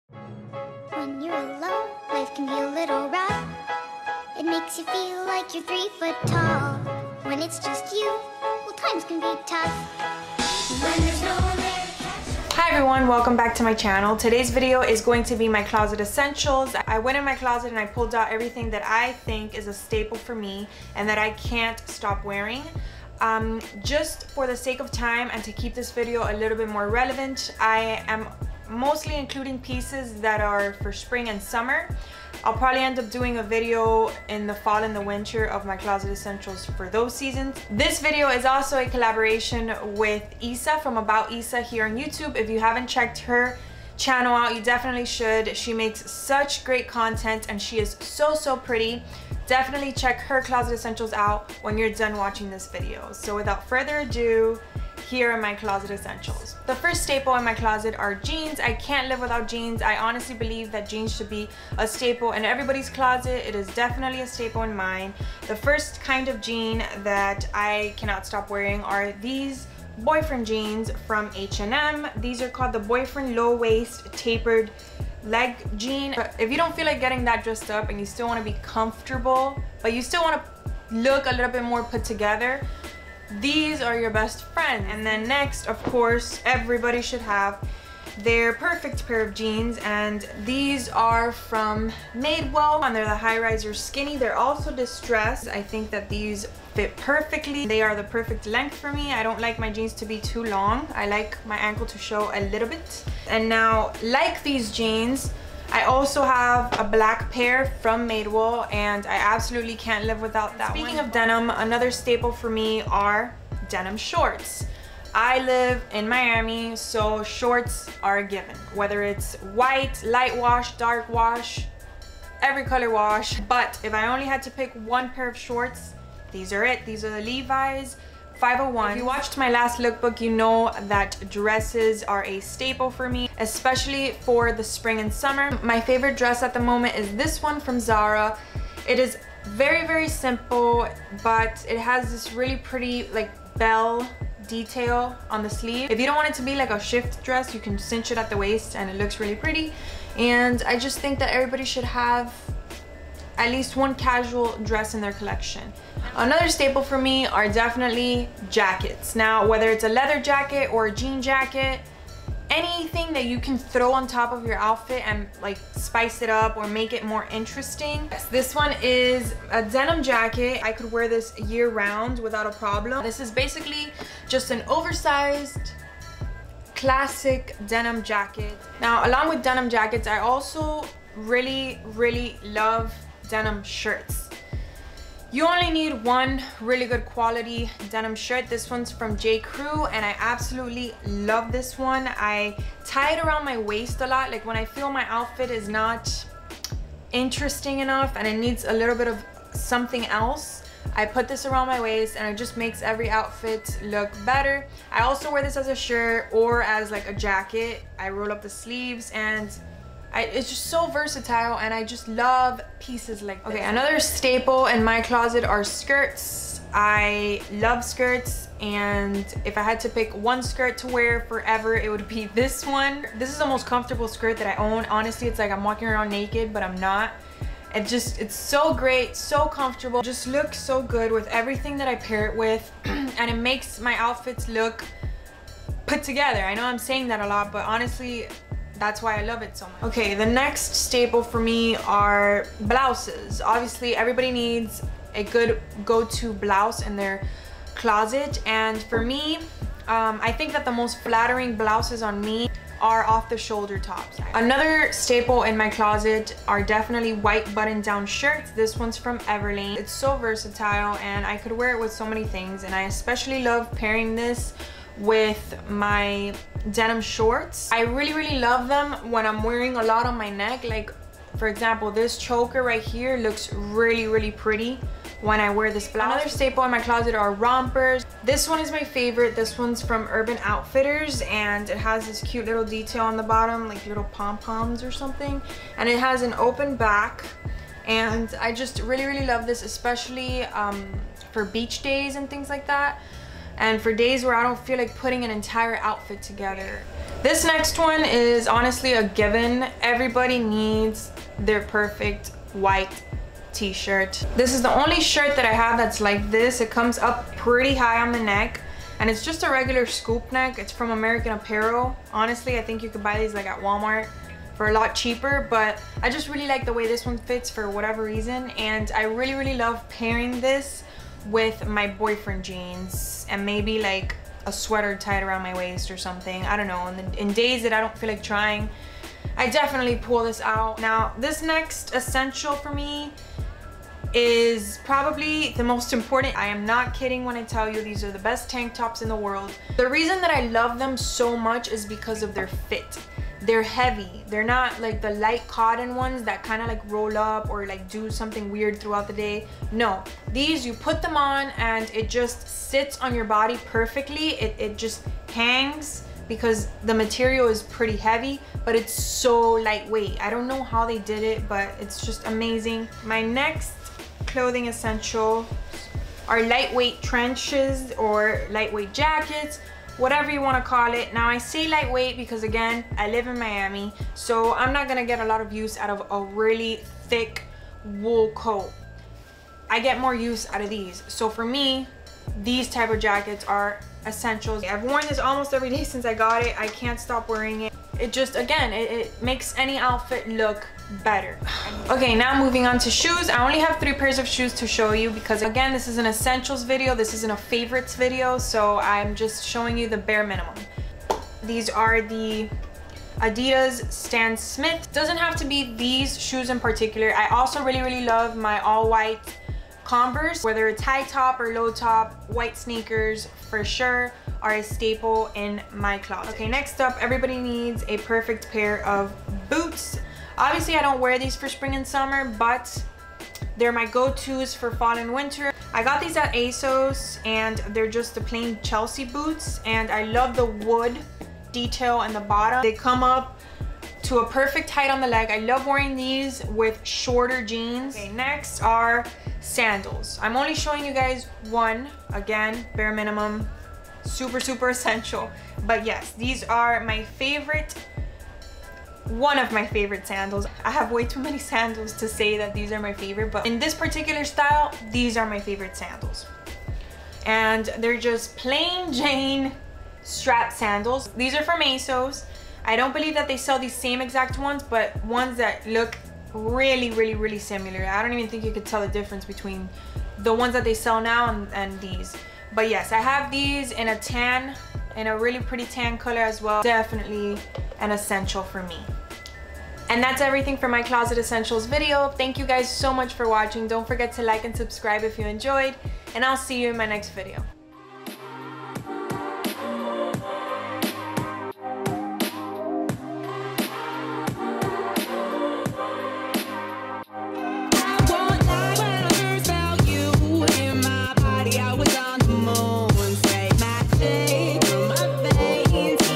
Closet Essentials feat. About Issa
Here are my Closet essentials.This video is a collaboration with the lovely Issa from About Issa, make sure to go and check her video out as well!

Originally this video was A LOT longer but I decided to really only include the clothing pieces that I truly cannot live without & that I think everyone should have in their closet.

PS. I'm currently on vacation in Europe. Add me on snapchat to watch me take on Italy, Croatia & Greece!

-SOCIAL-
IG & snapchat: Gemary